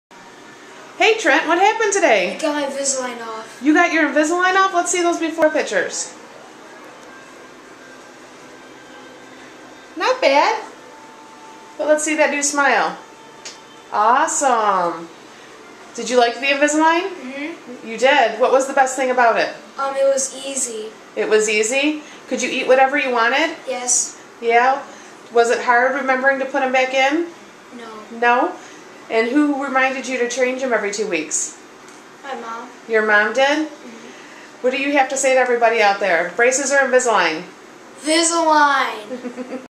Trent Tappan Finished Invisalign Treatment
Trent Tappan shares his experience with Invisalign.